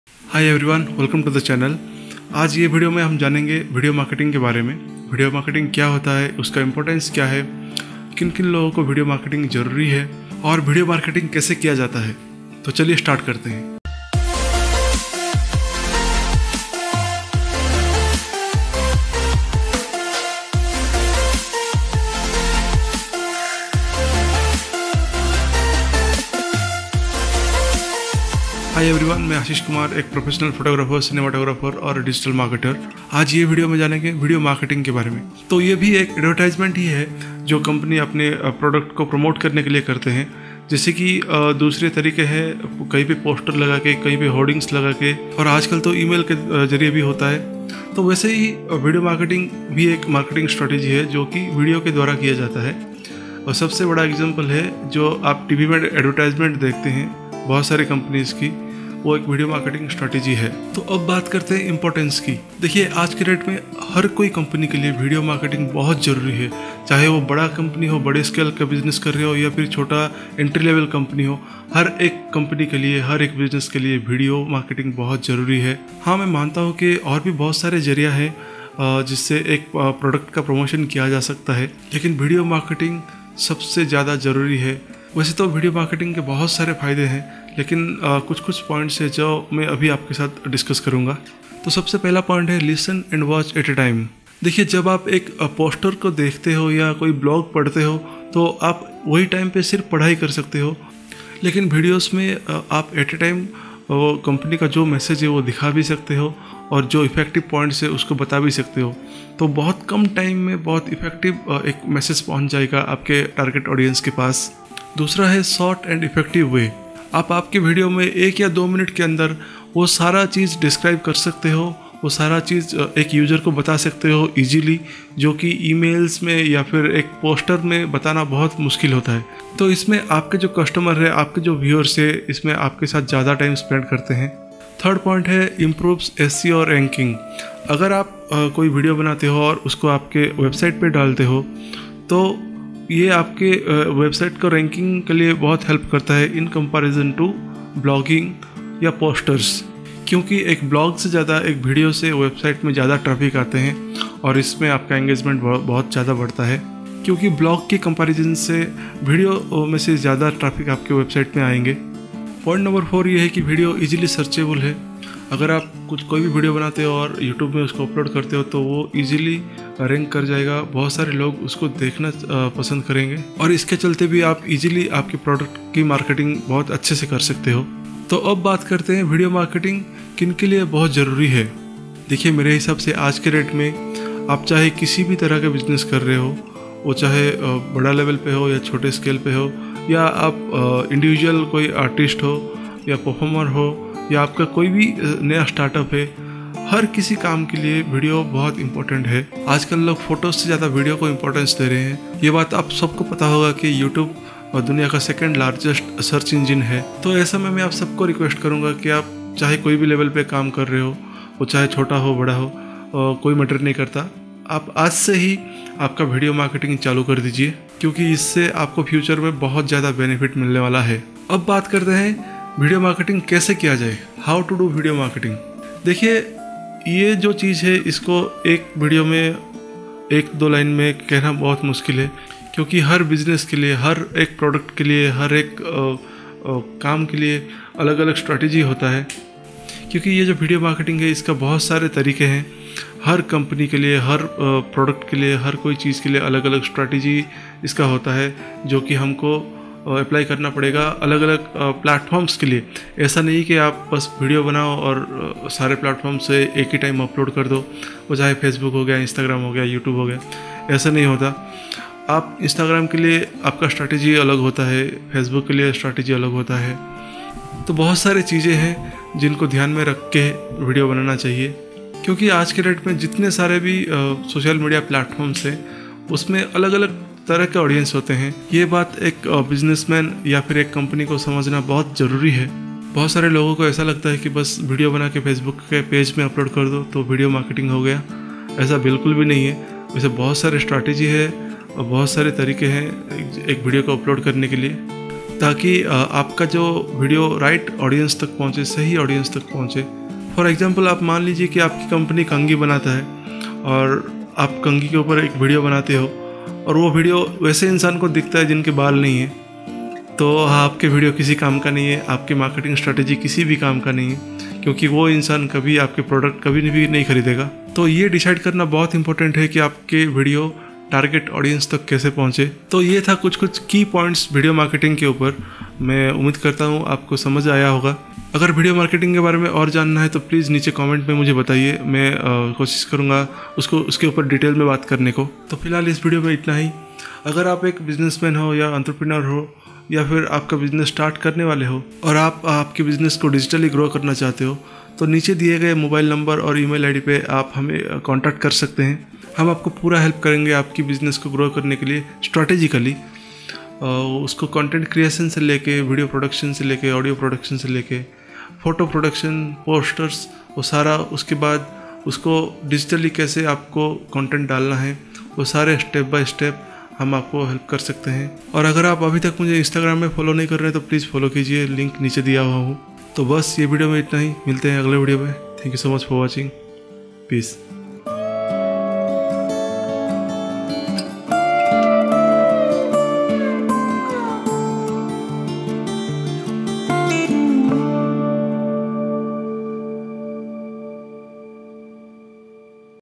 WHAT IS video marketing | HOW TO DO | IN HINDI | 2020| ASHIS KUMAR
Hi everyone, this video is all about video marketing and its importance and how to do video marketing.  Kindly watch the video till the end and leave your valuable comment.